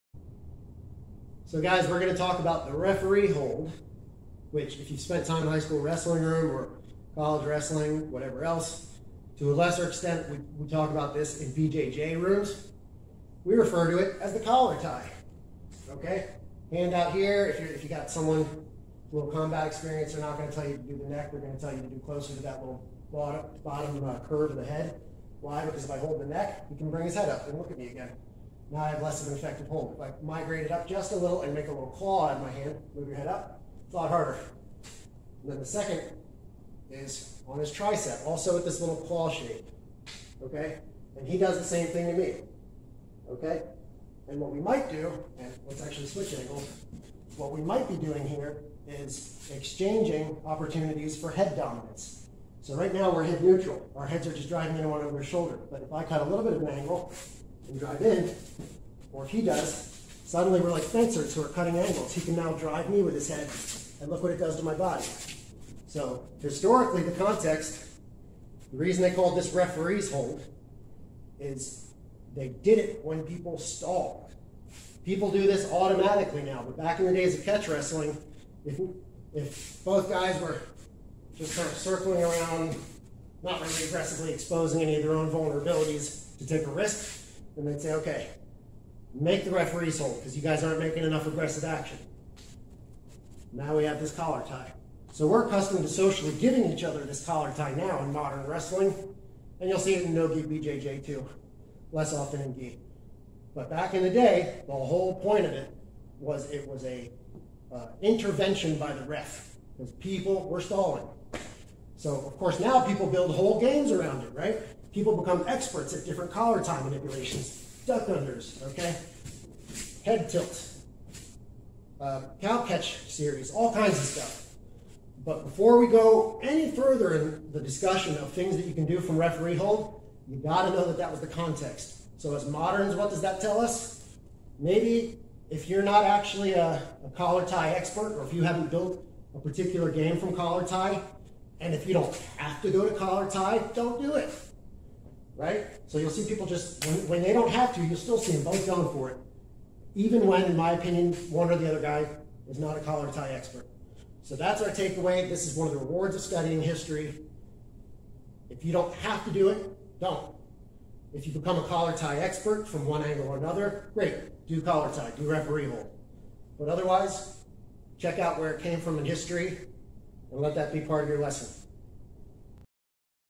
Historical Intro to the "Referee's Hold" (Collar Tie)
This video gives an extremely brief video overview of the historical origin of the "referee's hold" (collar tie) as described by the "Spalding's How To Wrestle" text from 1856; a classic reference text for historical catch wrestling.